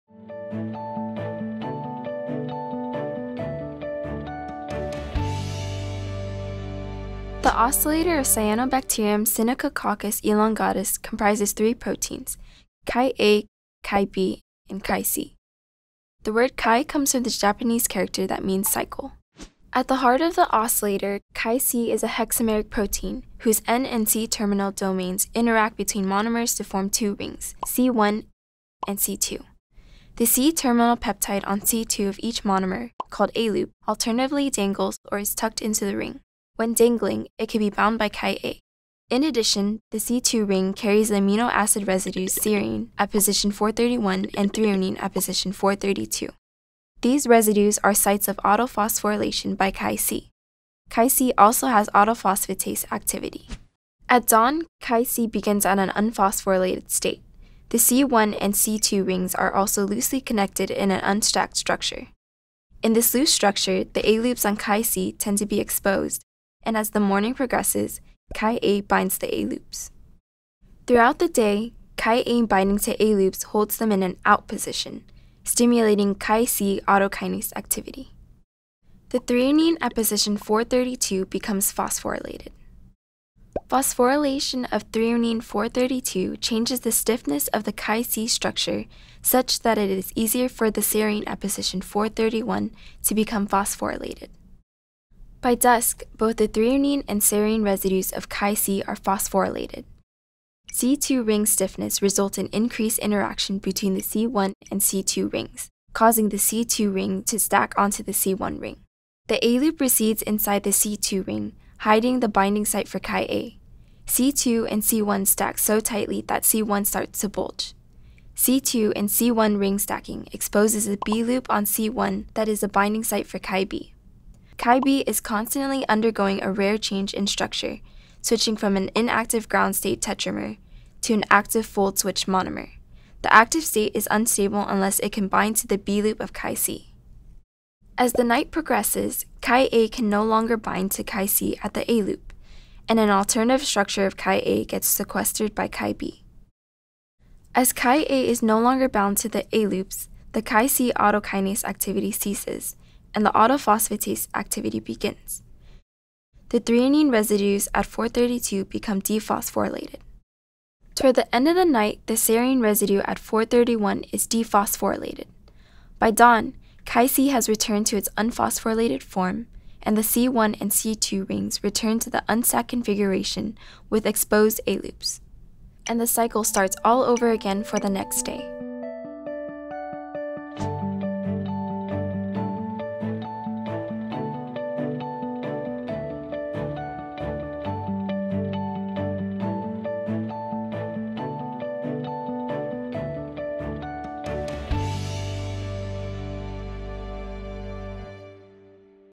Cyanobacterial Circadian Oscillator Animation
This animation demonstrates the circadian oscillator mechanism in the cyanobacterium Synechococcus elongatus. It was produced and developed by members of the BioClock Studio Winter 2016 at UC San Diego.